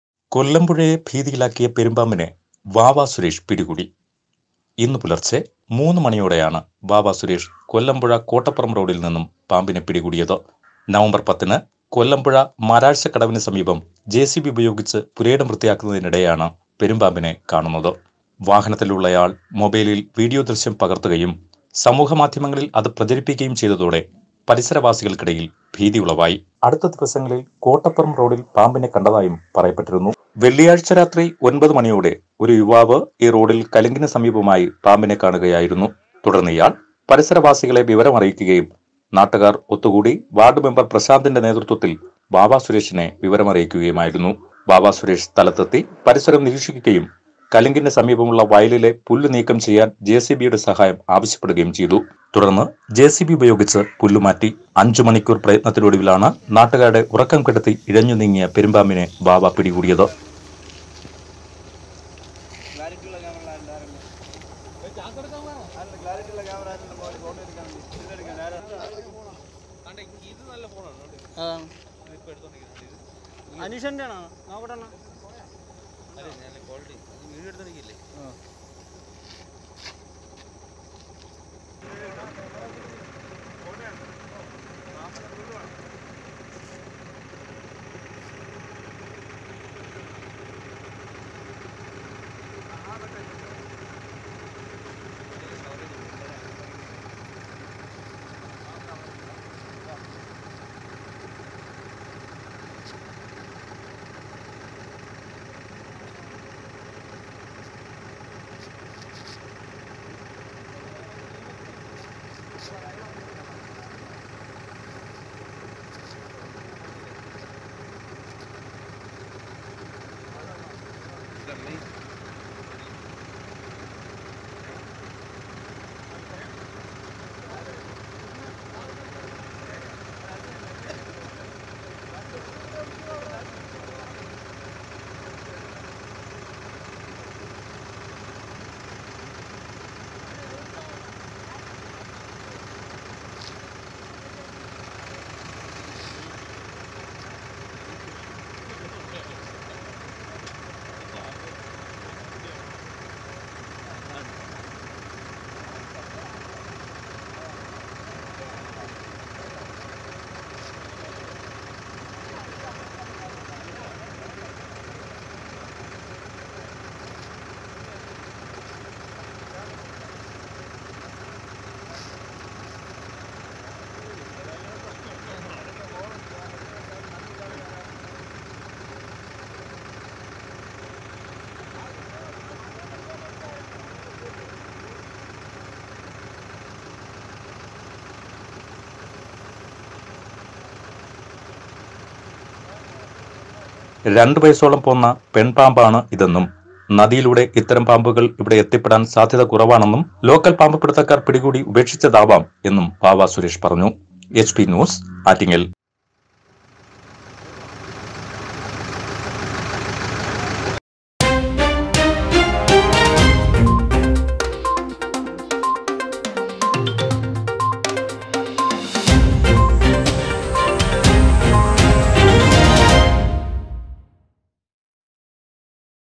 കൊല്ലമ്പുഴയെ ഭീതിയിലാക്കിയ പെരുംപാമ്പിനെ പിടികൂടുന്ന ദൃശ്യം
ആറ്റിങ്ങൽ: കൊല്ലമ്പുഴയെ ഭീതിയിലാക്കിയ പെരുംപാമ്പിനെ വാവ സുരേഷ് പിടികൂടി. ഇന്ന് പുലർച്ചെ 3 മണിയോടെയാണ് വാവ സുരേഷ് കൊല്ലമ്പുഴ കോട്ടപ്പുറം റോഡിൽ നിന്നും പാമ്പിനെ പിടികൂടിയത്. നവംബർ പത്തിനാണ് കൊല്ലമ്പുഴ മാരഴ്ച്ചകവിനുസമീപം ജെ സി ബി ഉപയോഗിച്ച് പുരയിടം വൃത്തിയാക്കുന്നതിനിടെ പെരുംപാമ്പിനെ കാണുന്നത്. വാഹനത്തിലുള്ളയാൾ മൊബൈലിൽ വീഡിയോ ദൃശ്യം പകർത്തുകയും സമൂഹ മാധ്യമങ്ങളിൽ അത് പ്രചരിക്കുകയും പരിസരവാസികൾക്കിടയിൽ ഭീതി ഉളവാക്കുകയും ചെയ്തിരുന്നു. എന്നാൽ അടുത്ത ദിവസങ്ങളിൽ കോട്ടപ്പുറം റോഡിൽ പാമ്പിനെ കണ്ടതായും പറയപ്പെട്ടിരുന്നു. വെള്ളിയാഴ്ച്ച രാത്രി ഒൻപത് മണിയോടെ ഒരു യുവാവ് ഈ റോഡിൽ കലിങ്കിന് സമീപമായി പാമ്പിനെ കാണുകയായിരുന്നു. തുടർന്നിയാൾ പരിസരവാസികൾ വിവരമറിയിക്കുകയും നാട്ടുകാർ ഒത്തുകൂടി വാർഡ് മെമ്പർ പ്രശാന്തിന്റെ നേതൃത്വത്തിൽ വാവ സുരേഷിനെ വിവരമറിയിച്ചു. വാവസുരേഷ് സ്ഥലത്തെത്തി പരിസരം നിരീക്ഷിക്കുകയും കലിങ്കിന്റെ സമീപമുള്ള വയലിലെ പുല്ല് നീക്കം ചെയ്യാൻ ജെ സി ബിയുടെ സഹായം ആവശ്യപ്പെടുകയും ചെയ്‌തു. ജെ സി ബി ഉപയോഗിച്ച് പുല്ല് മാറ്റുന്നതിനിടയിൽ അഞ്ചുമണിക്കൂറിലെ പ്രയത്നത്തിലൊടുവിൽ നാട്ടുകാരുടെ ഉറക്കംകെടുത്തി ഇഴഞ്ഞു നീങ്ങിയ പെരുംപാമ്പിനെ വാവ പിടികൂടി. രണ്ട് വയസോളം പോന്ന പെൺ പാമ്പാണിതെന്നും നദിയിലൂടെ ഇത്തരം പാമ്പുകൾ ഇവിടെ എത്തിപ്പെടാൻ സാധ്യത കുറവാണെന്നും, ലോക്കൽ പാമ്പ് പിടുത്തക്കാർ പിടികൂടി ഉപേക്ഷിച്ചതാവാം എന്നും വാവ സുരേഷ് പറഞ്ഞു.